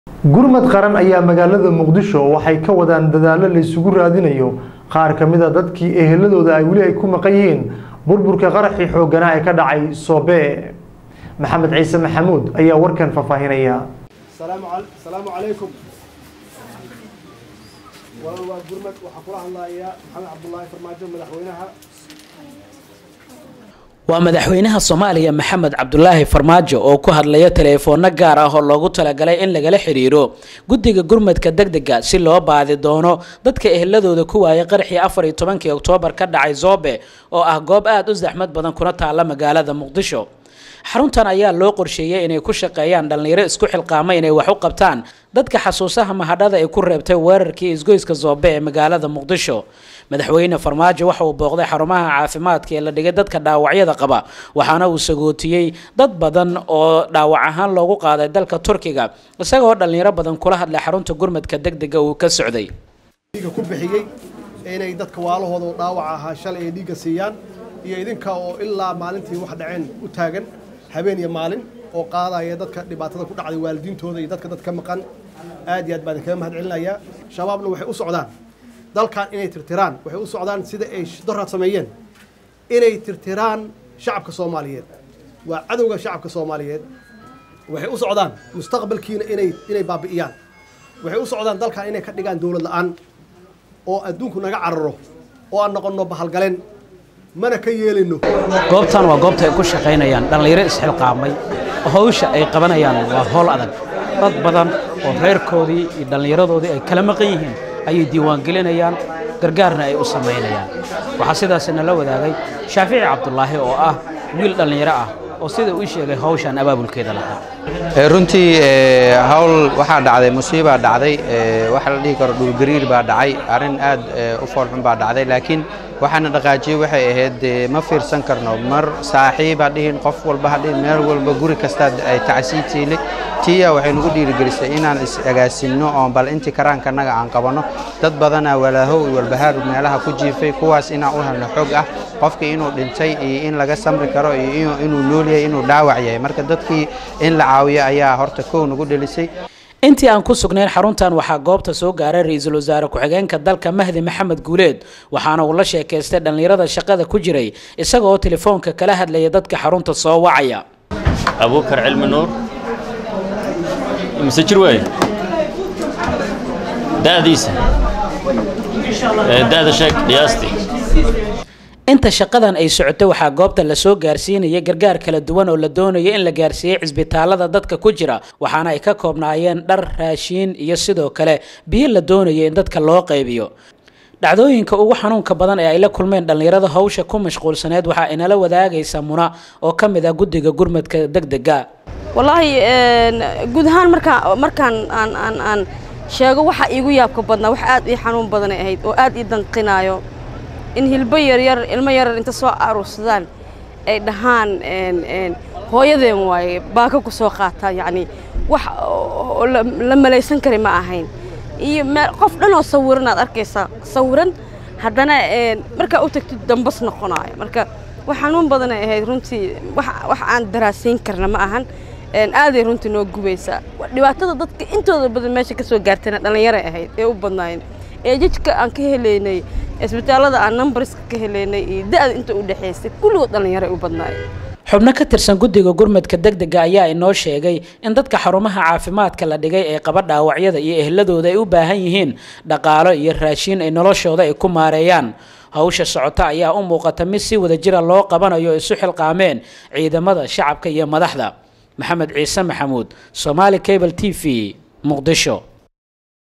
Gurmad qaran oo uu qeyb ka yahay madaxweynaha Soomaaliya ayaa ka socda Muqdisho
Gurmad qaran oo uu qeyb ka yahay madaxweynaha Soomaaliya Maxamed Cabdulaahi Farmaajo ayaa ka socda Muqdisho